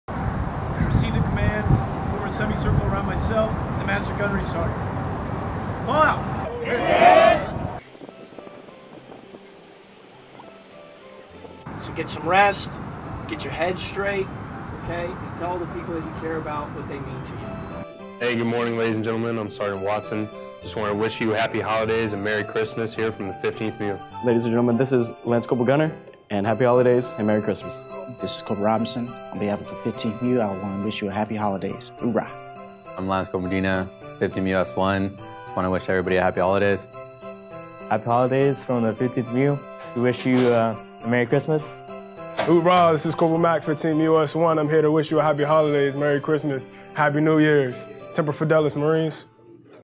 Holiday Greetings
Credit: Lance Cpl. Nathan McAllister | Date Taken: 12/12/2016
Season's greetings from the 15th Marine Expeditionary Unit, we wish you and yours happy holidays.